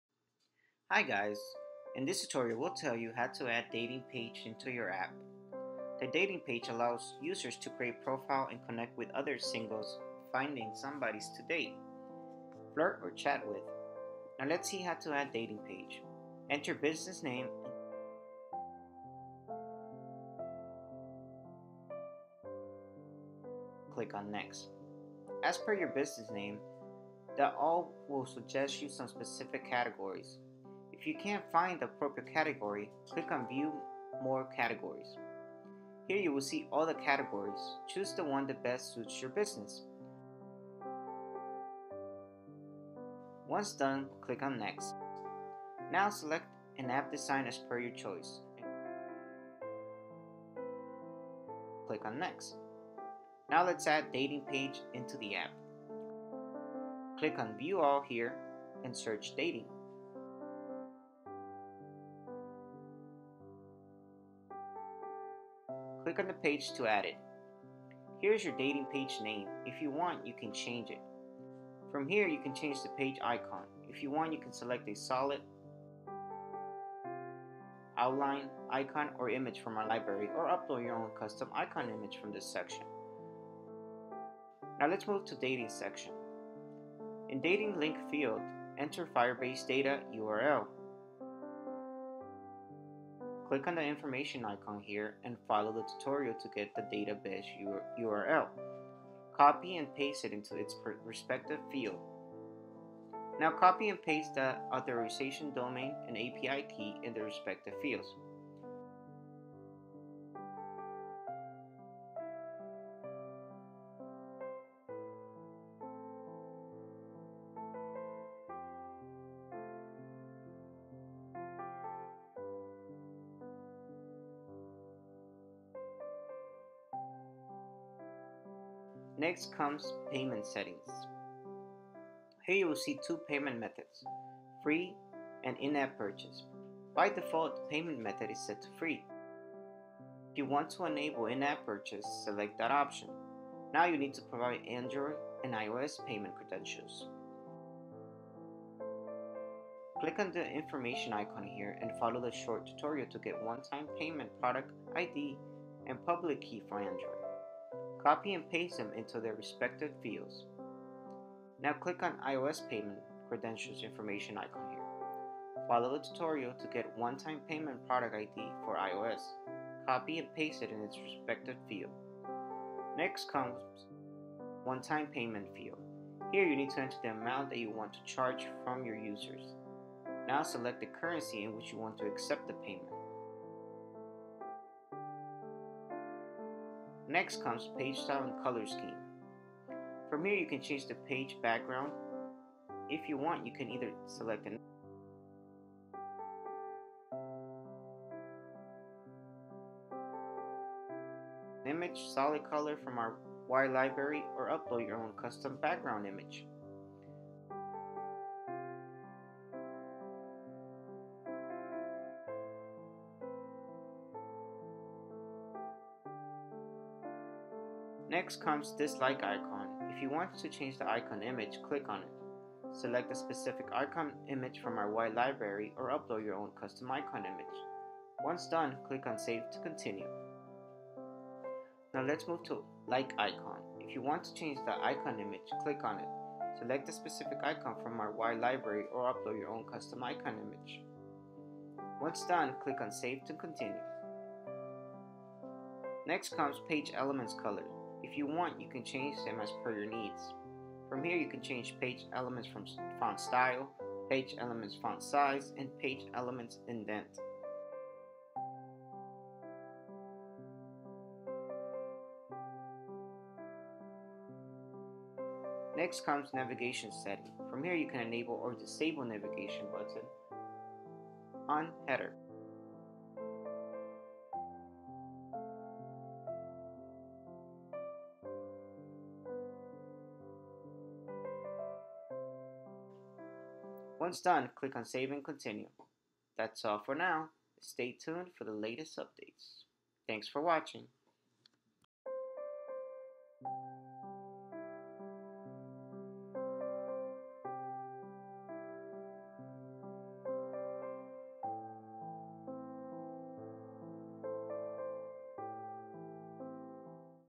How to create a dating app?
This video will help you,How you can add dating page .